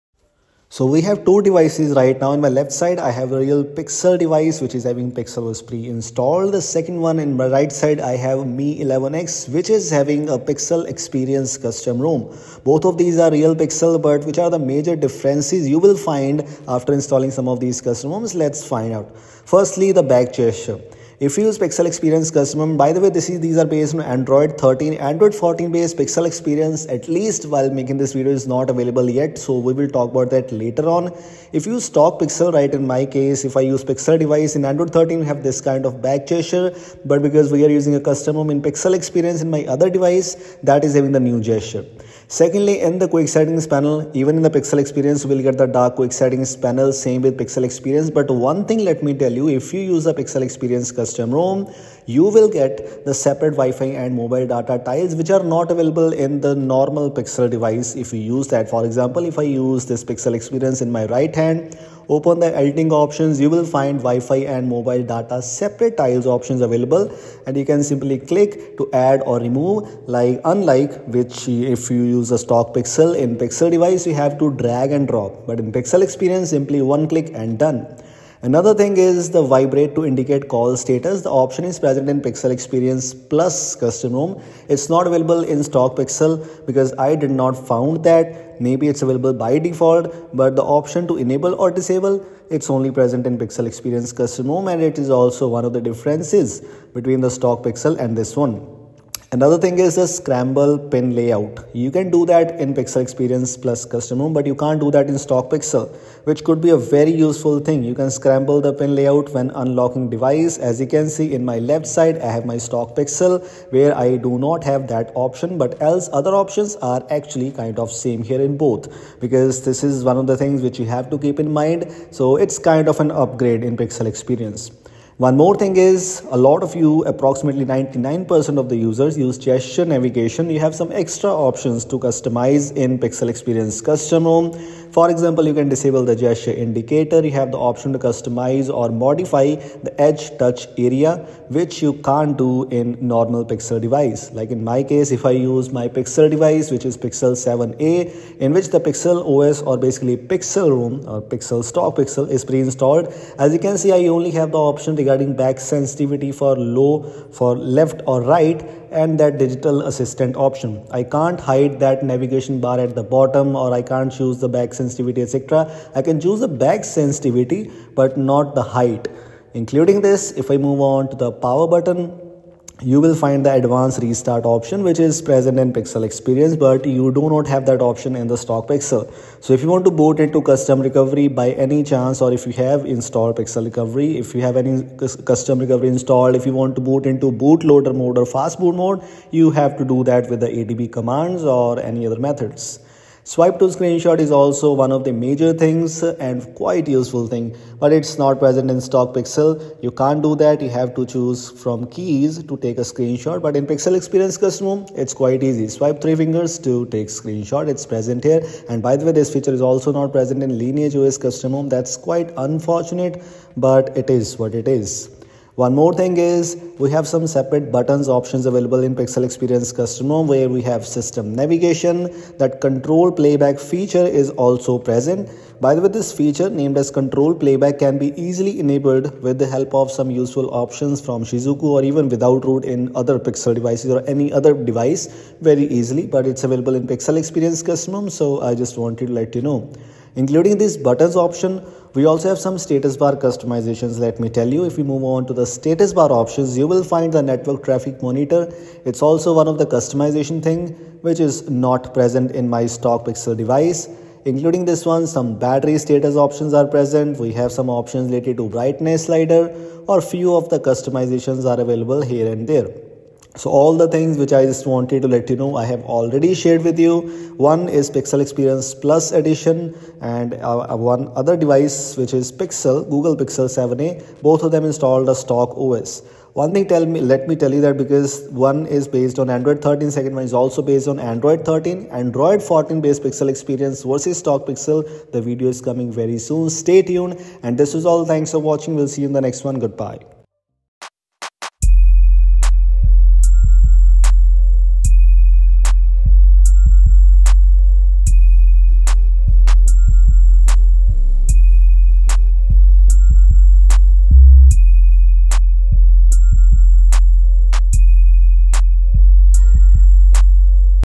Pixel Experience VS Stock Pixel - 5 Key Differences to Check out: 🤩
Hey guys, What's Up? Everything is good I Hope. In this video, I'm going to show you " Pixel Experience VS Stock Pixel - 5 Key Differences to Check out: 🤩 ". Make Sure to watch this video till the end to understand everything.